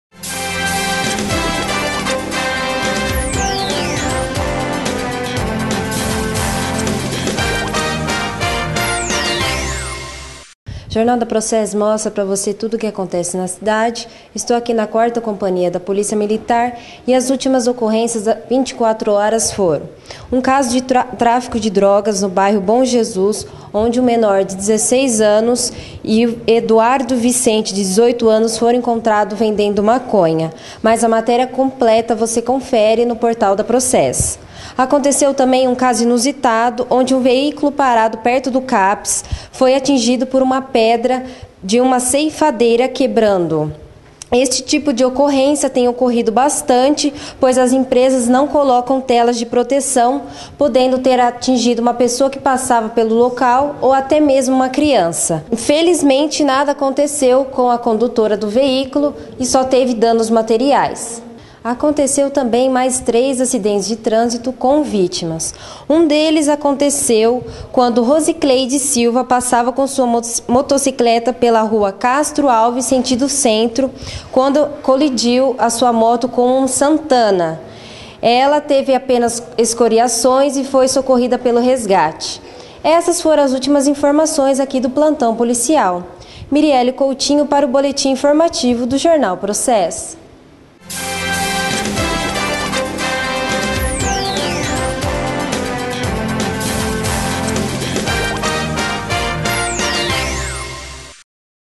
process.com.br - Boletim de ocorrencias - 260712
Plantão das últimas noticias desta quinta-feira (26)

Caso de Tráfico de Drogas, 3 acidentes de trânsito com vítimas e um dano de veículo. Confira os detalhes:

Plantão policial com poucas ocorrências de vulto. Um caso de Tráfico de Drogas no bairro Bom Jesus, onde um jovem de 16 anos e Eduardo Vicente de 18 anos, foram encontrados comercializando maconha, essa matéria completa já foi publicada aqui no Portal Process.

Aconteceu um caso inusitado onde um veículo estava parado perto do CAPS (Centro de Atendimento Psicossocial) de Matão, quando teve o vidro lateral atingido por uma pedra de uma ceifadeira, quebrando-o. Esse tipo de ocorrência vem acontecendo com frequência, pois a empresa Leão Ambiental, via de regra, não coloca a tela de proteção. O fato aconteceu danificando um veículo, mas poderia ter atingido uma criança ou alguém que passava por ali, no momento. Felizmente nada aconteceu com a condutora do veículo, Janaina F. Serenoni, tendo apenas danos materiais.

Mais três acidentes de trânsito, infelizmente todos com vítimas, um deles aconteceu quando Rosicleide Silva trafegava com sua motocicleta pela Rua Castro Alves sentido Centro -- bairro, quando um Santana veio a colidir com sua moto, ela teve escoriações e foi socorrida pela Unidade de Resgate do Corpo de Bombeiros.

Confira os detalhes e as imagens na reportagem de Mirieli Coutinho.

Autora da Matéria:
Mirieli Coutinho